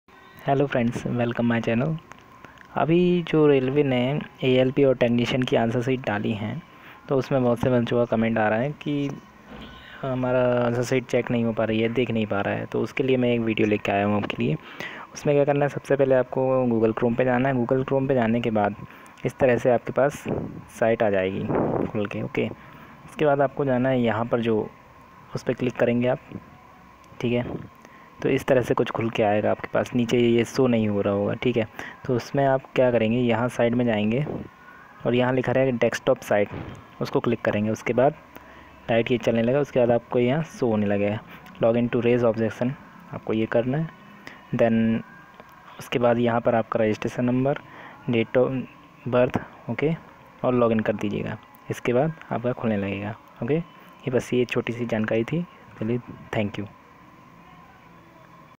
Agar nhi khul rhi h alp/technician ki Answer sheet to video dekhe
alp/technician ki answer sheet aese khole